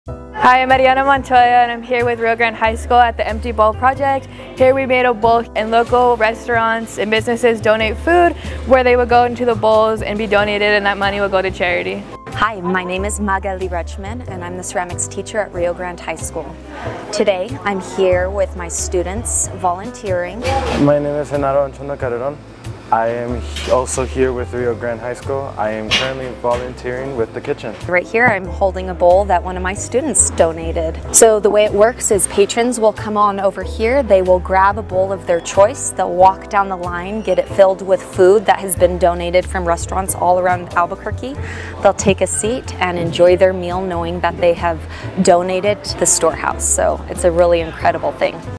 Rio Grande High students DONATE their handmade bowls and VOLUNTEER at the Empty Bowl Project!
Rio Grande Ceramics students not only donated their beautiful hand made bowls to the Empty Bowl Project, they are also out there VOLUNTEERING their time with their teacher Magali R. APS Fine Arts students are the best, like if you agree!

A special shout out to the students from the Cibola High Jazz Band (performing in their smaller combo called 'Burgundy Blue'), who played at this event too, AND are headed off to the ZIA Marching Band fiesta shortly after to perform with their school's marching band. WOW! Let's show them some support!